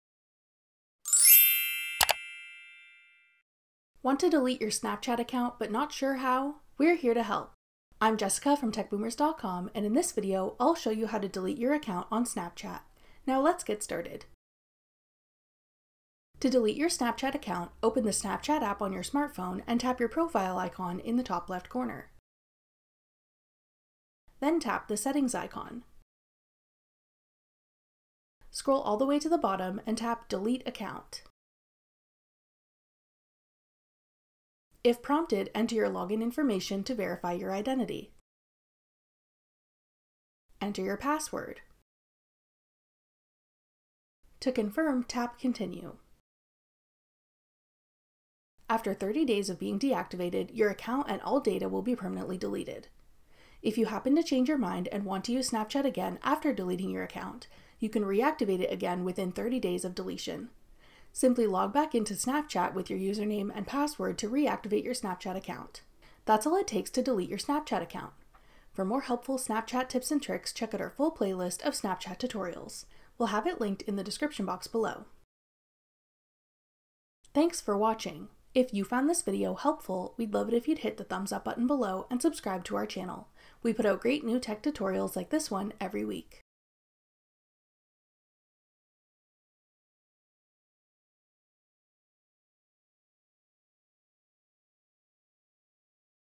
How to Delete a Snapchat Account (Full Guide)
Want to delete your Snapchat account but not sure how? Watch this video to learn how to quickly & easily delete your account on Snapchat.

To delete your Snapchat account, open the Snapchat app on your smartphone and tap your profile icon in the top left corner. Then tap the Settings icon. Scroll all the way to the bottom and tap Delete Account. If prompted, enter your login information to verify your identity. Then tap Delete My Account. Enter your password. To confirm, tap Continue. After 30 days of being deactivated, your account and all data will be permanently deleted.

If you happen to change your mind and want to use Snapchat again after deleting your account, you can reactivate it again within 30 days of deletion. Simply log back into Snapchat with your username and password to re-activate your Snapchat account.